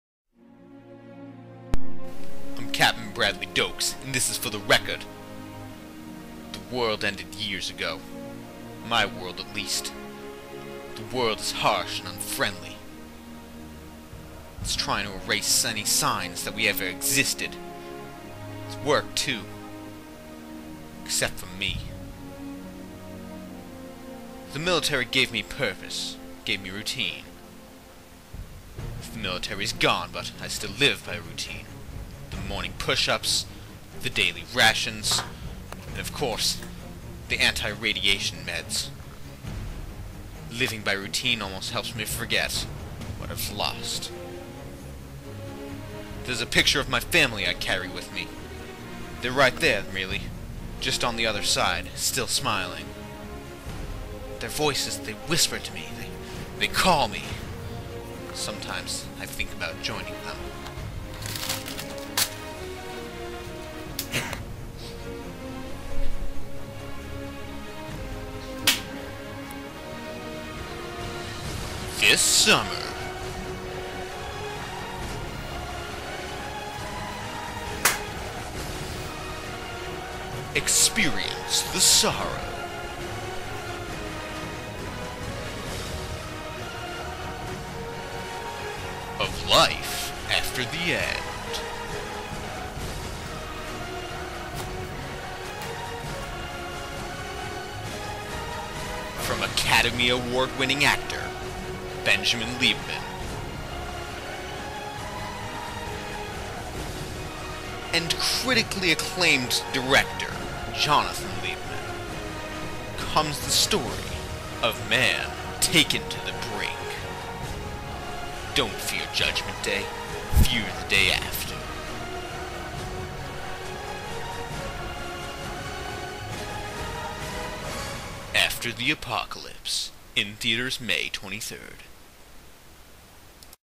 After the Apocalypse Trailer
Final project for my brother's English 10 class. He wrote, filmed, and directed the trailer; I acted for it and did the voiceover. Why I allow such fantastic acting to remain public is anyone's guess